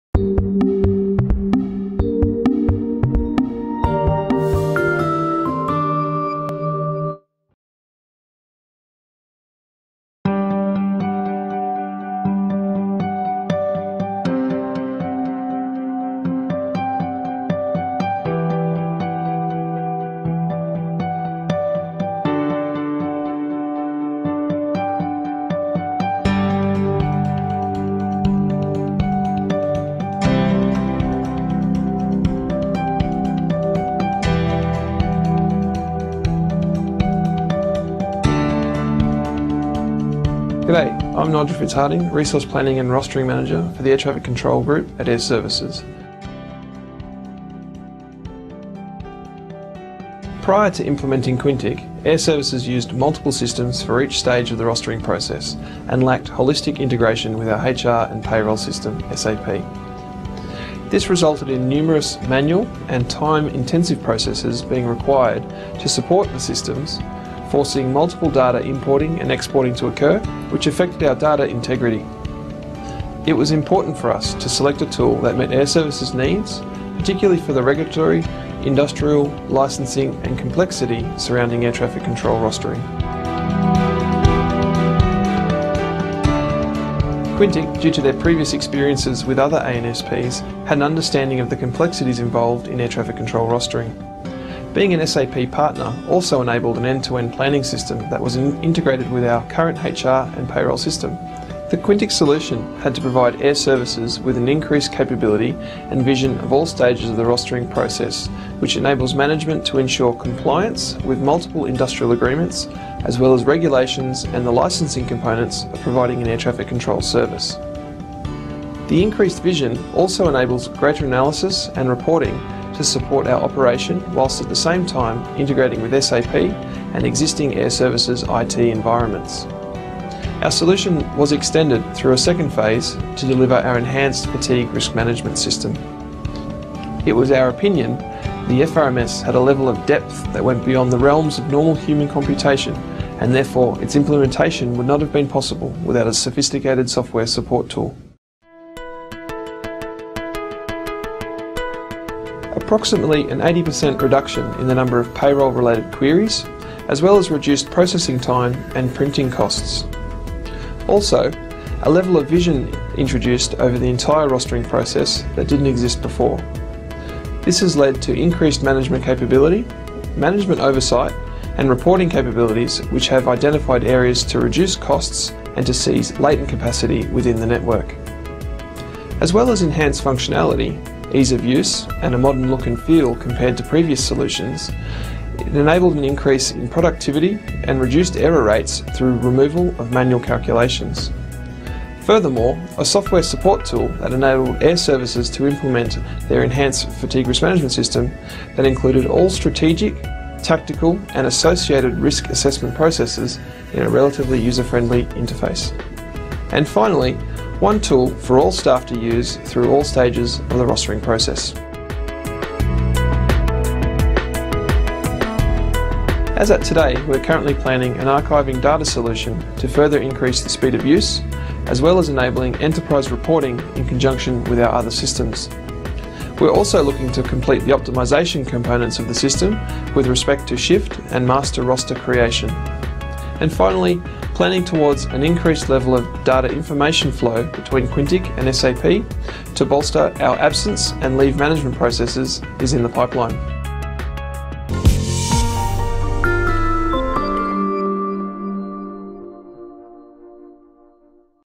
Air Traffic Control Rostering - DELMIA for Airservices
Meet Airservices' Resource Planning & Rostering Manager, Nigel Fitzhardinge, as he describes the DELMIA Quintiq solution to Airservices' planning puzzles. In this video, Nigel outlines the improvements seen at Airservices since moving from multiple systems to DELMIA Quintiq - one planning system for all planning puzzles, fully integrated (including with SAP). He also discusses DELMIA Quintiq's fatigue risk management capabilities, as well as future uses for DELMIA Quintiq at Airservices.